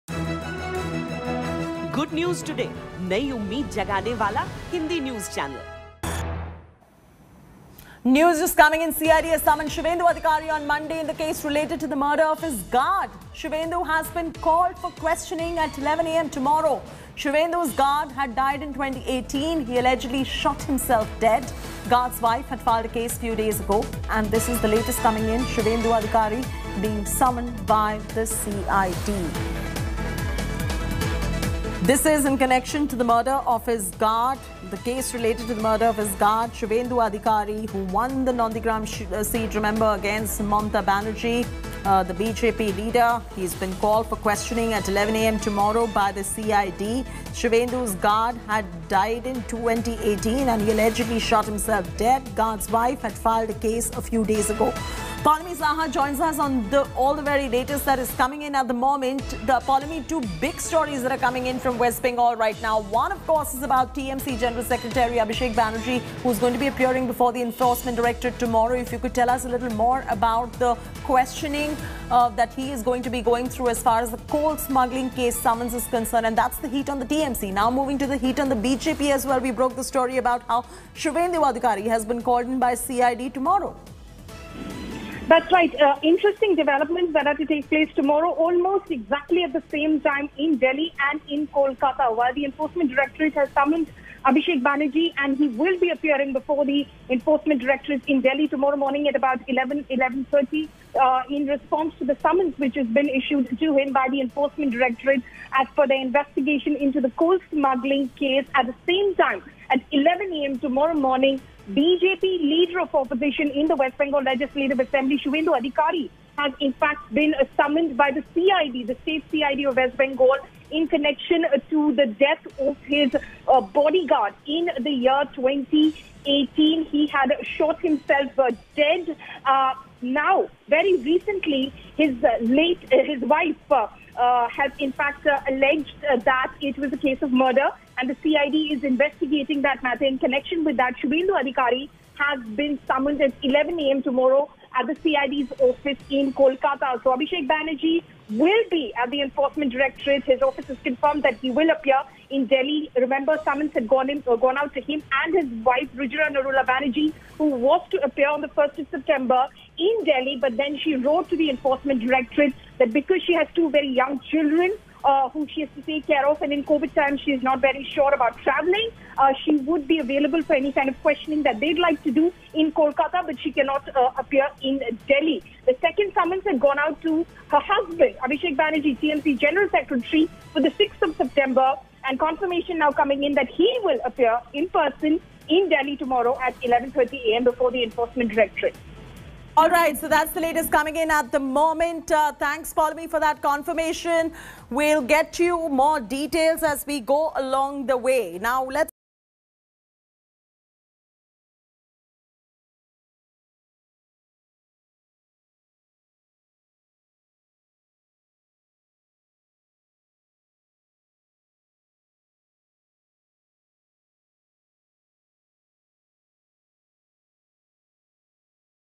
West Bengal CID Summons Suvendu Adhikari In His Bodyguard's Death Case | Breaking News
BJP leader Suvendu Adhikari has been summoned by West Bengal CID in connection to the investigation of the case of his bodyguard Subhabrata Chakraborty's death. The West Bengal's leader of opposition (LoP) has been asked to appear at the CID headquarters in Bhawani Bhavan in Kolkata on Monday at 11 AM.